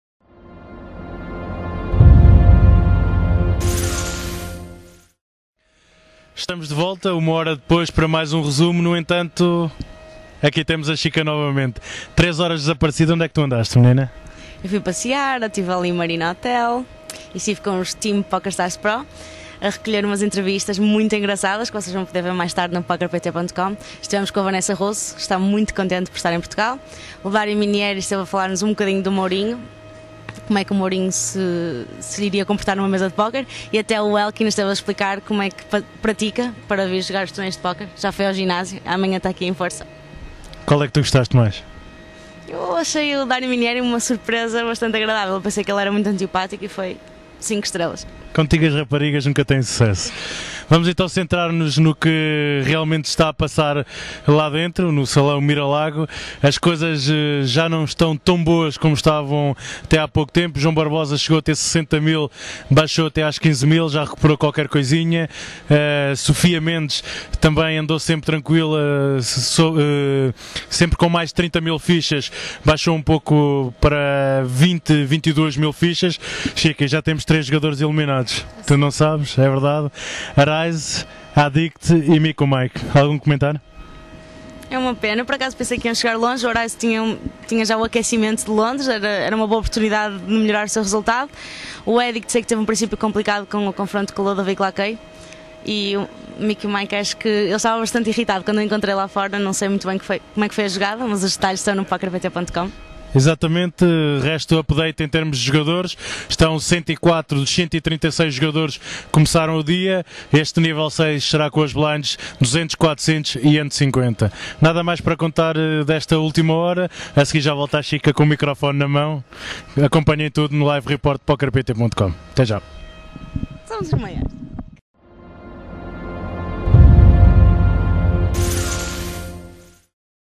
Final do Nível 6 do Dia 1A no EPT Vilamoura
- João Jomané Nunes e Vanda Xika Silva após o final do Nível 6